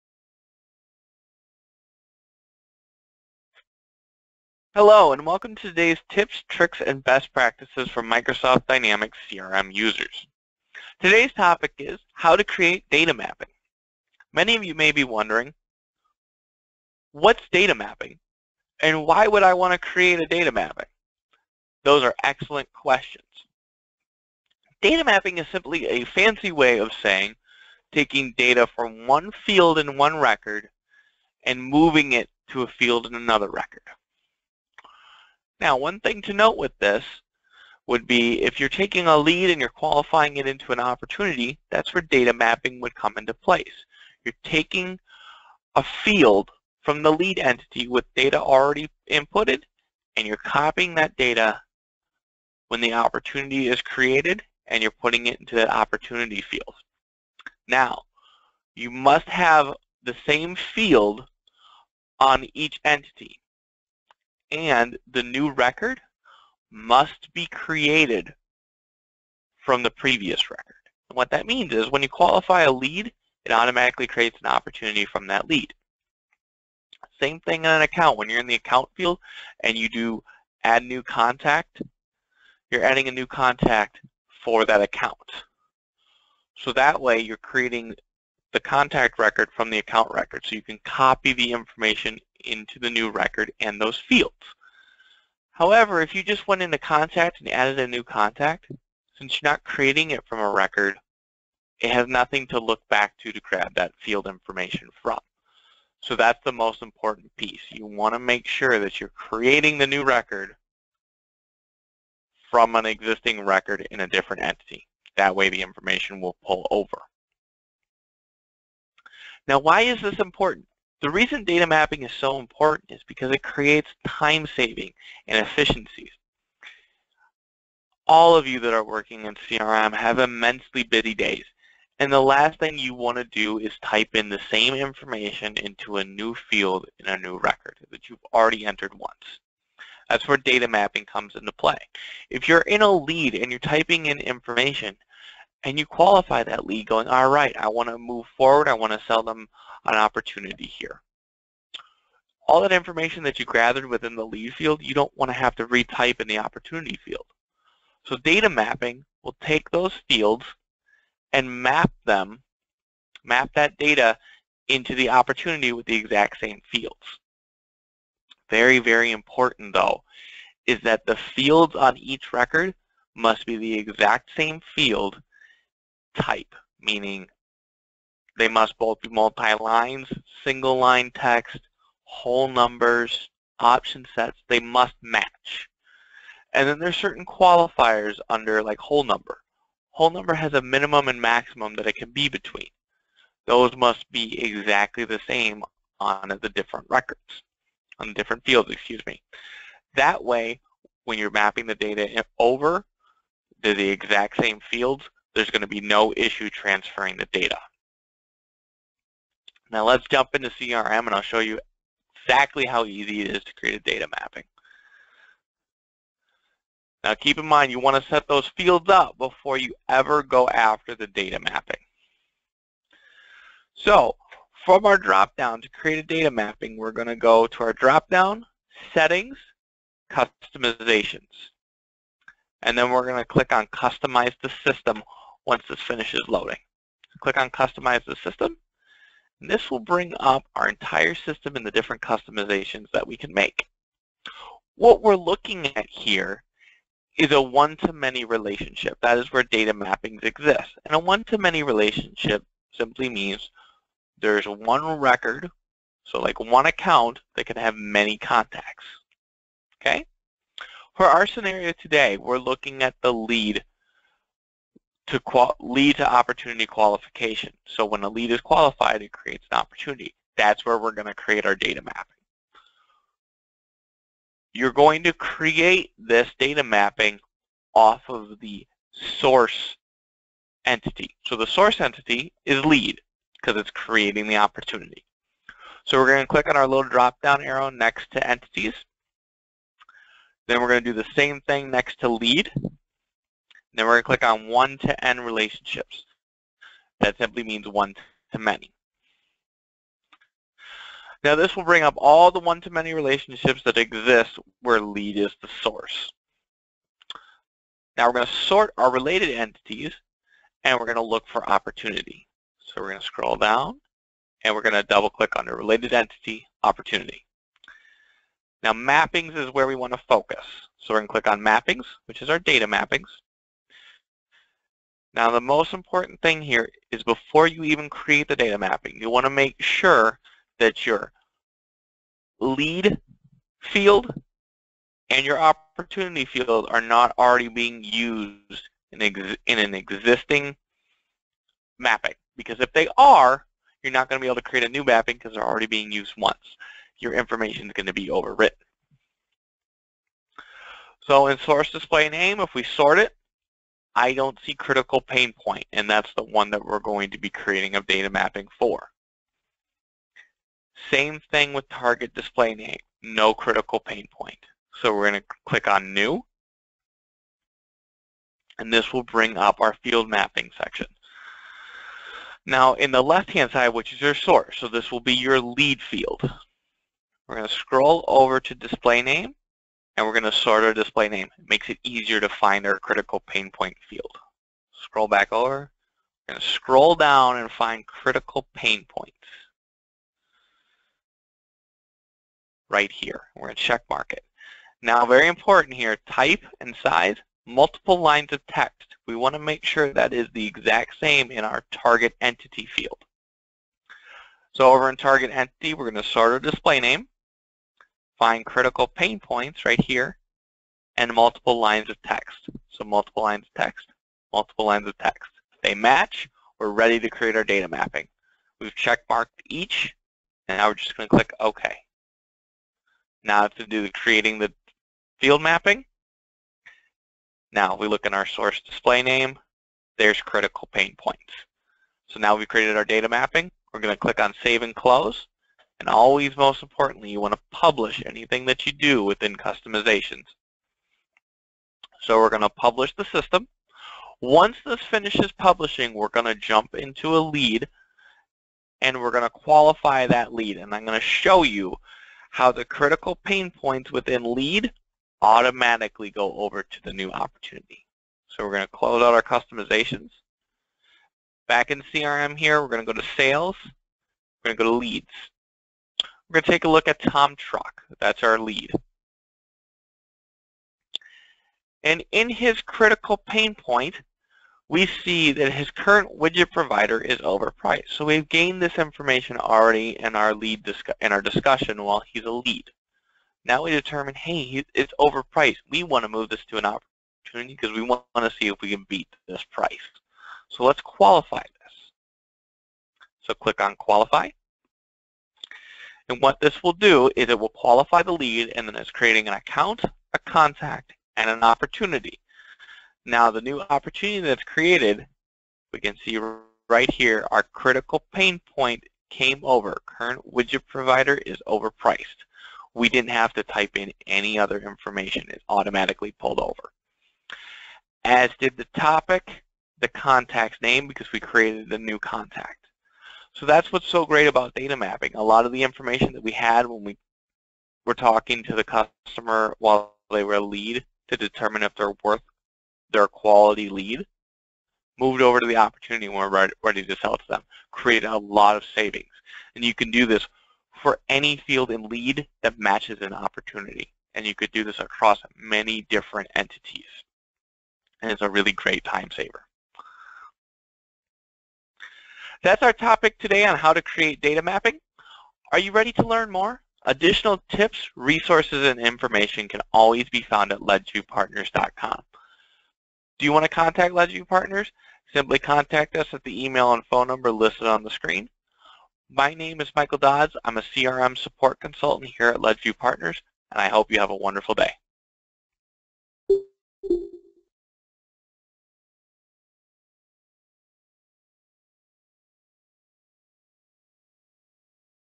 How to Create Data Mapping in Dynamics CRM
Learn how to create and use data mappings within Microsoft Dynamics CRM